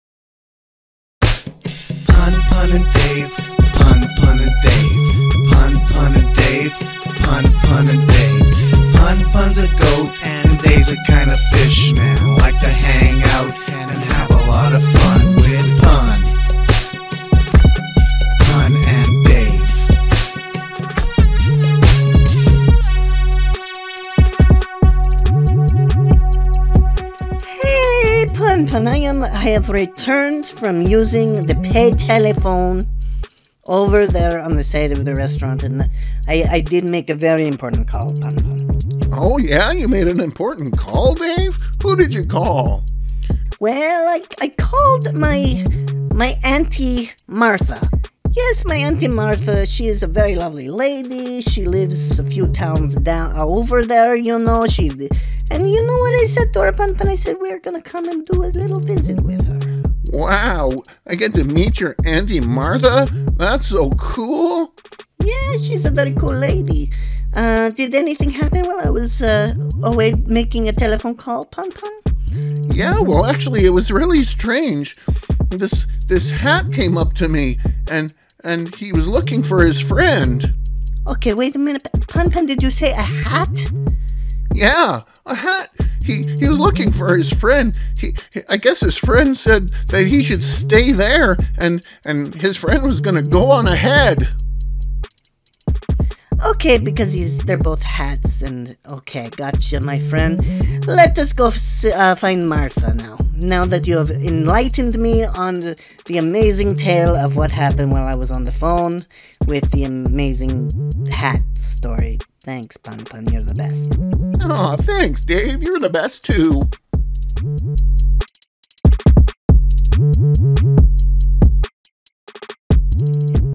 The Pun Pun and Dave Comedy Hour: Episode 12
A Very Lovely Lady

Pun Pun's a Goat
Dave's a kind of Fish Man
They like to hang out and have a lot of fun.

The best 1 or 2 minutes of Pun Pun and Dave you will see this week!